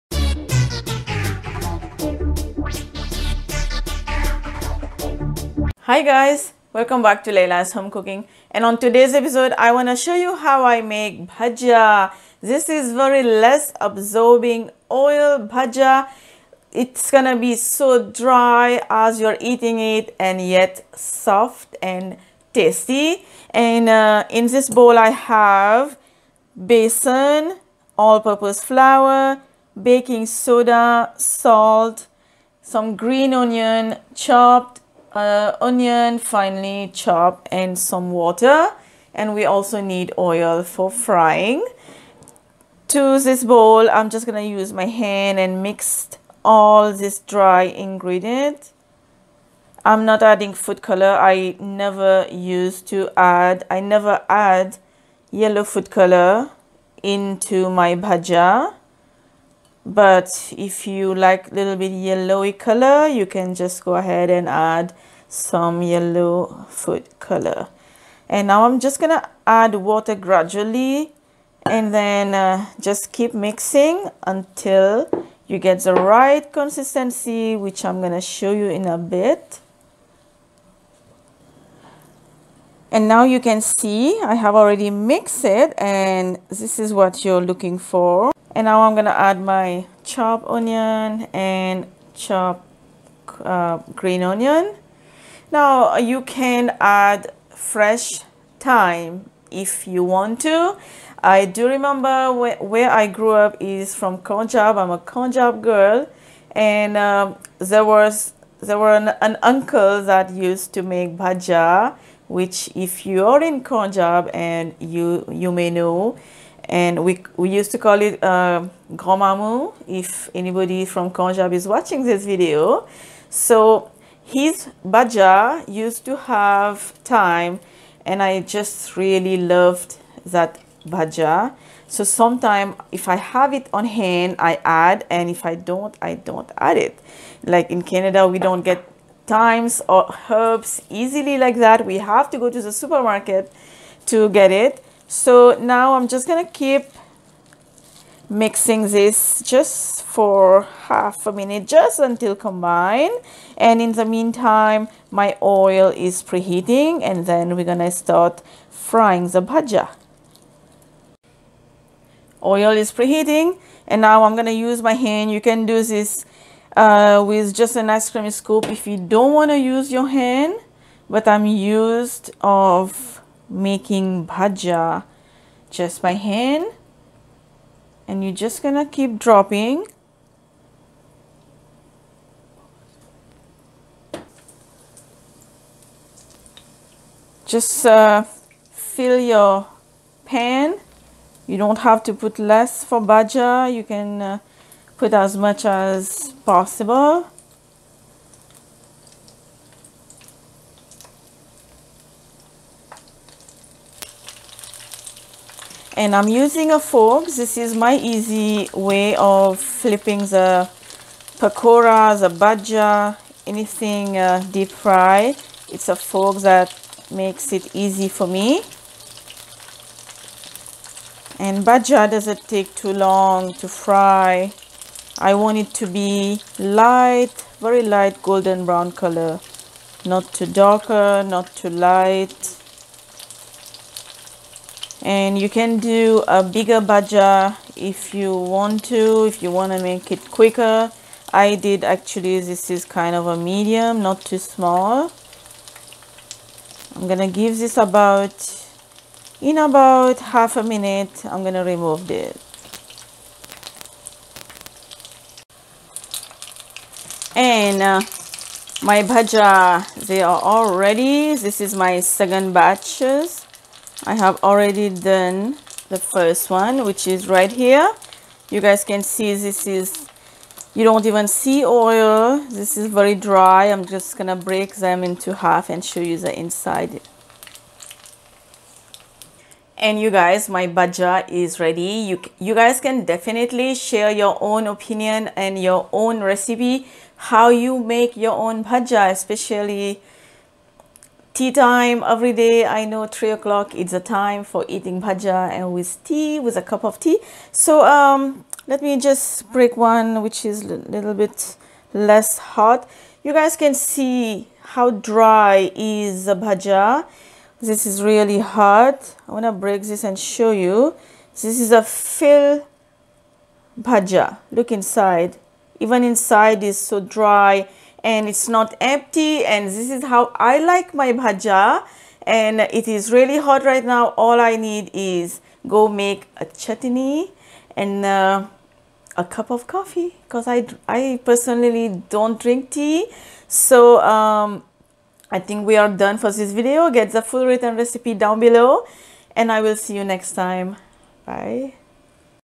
Bhajia (Pakora) without absorbing oil - Laila's Home Cooking - Episode 80
Ingredients:
340 grams of besan /or you can use 1 1/2 cup of besan
100 grams of all purpose flour/or use 3/4 cup of all purpose flour
1.35 ml of baking soda or a bit more than 1/4 teaspoon
150 grams of chopped green onions
1 medium sized onion finely chopped

Preparation

Heat oil in a deep dish pan on medium heat. In a bowl add flour, besan, salt and baking soda mix gradually add water mix until becomes a batter add green onions and chopped onions mix again and shape into small balls directly to the hot oil fry for 2-3 minutes or until light golden brown.

Recipe by Laila Jugon